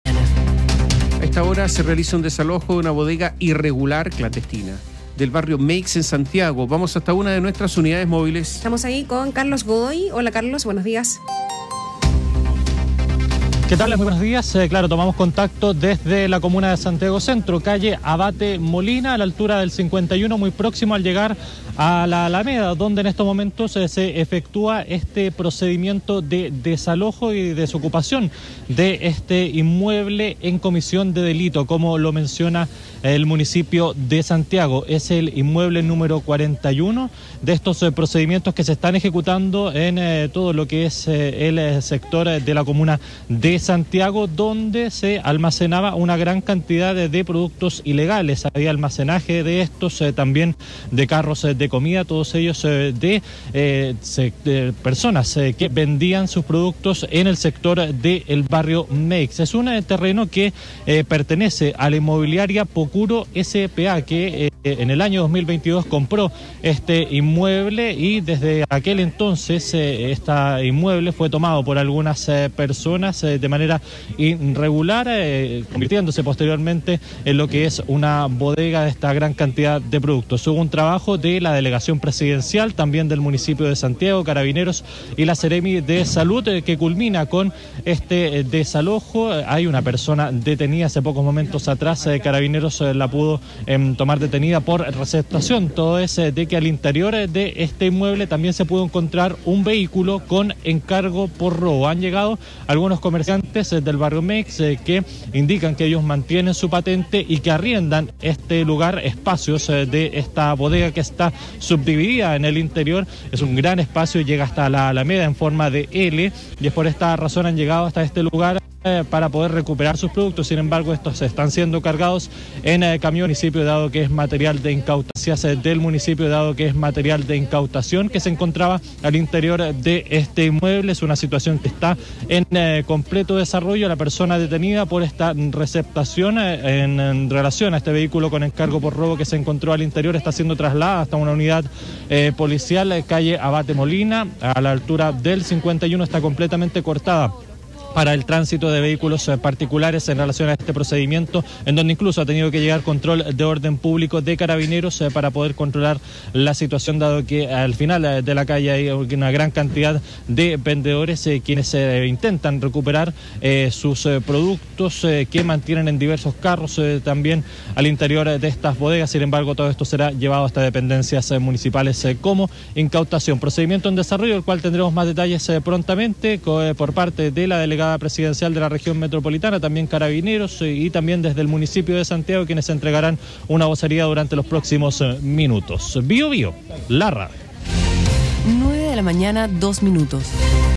Desalojan propiedad tomada en Meiggs que servía como bodega de ambulantes: hay un detenido
Se realizó el desalojo de una propiedad tomada en el barrio Meiggs de Santiago, que servía como bodega. Al interior se guardaban diversos productos, como también carros utilizados por los vendedores ambulantes.